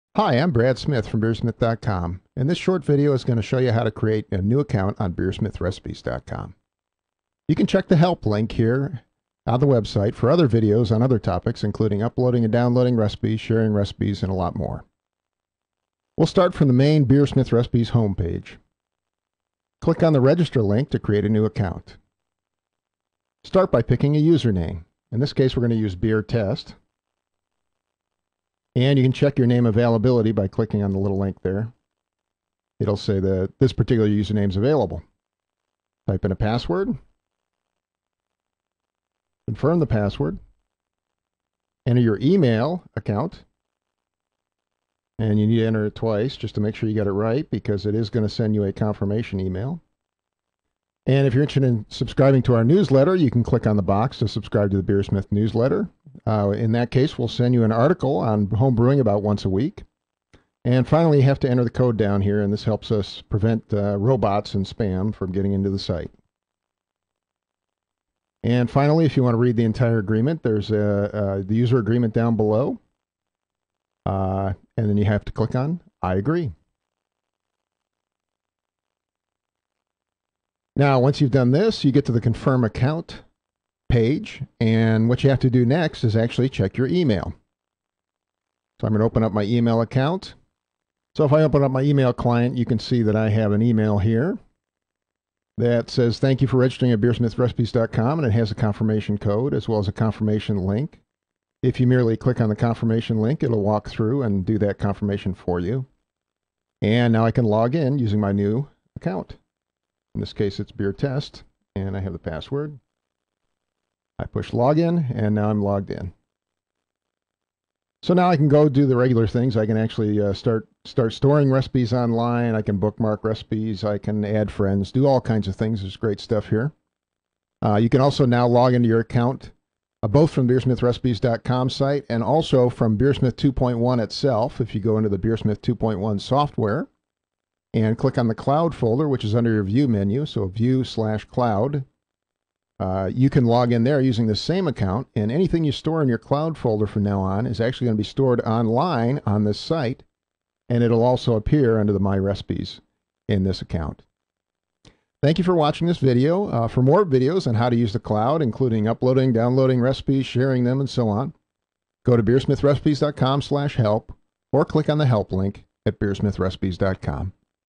Creating a BeerSmith Cloud Account
This tutorial walks you through creating an account on BeerSmithRecipes.com to use with the new recipe sharing site and recipe cloud features in BeerSmith 2.1 home brewing software to publish and share recipes.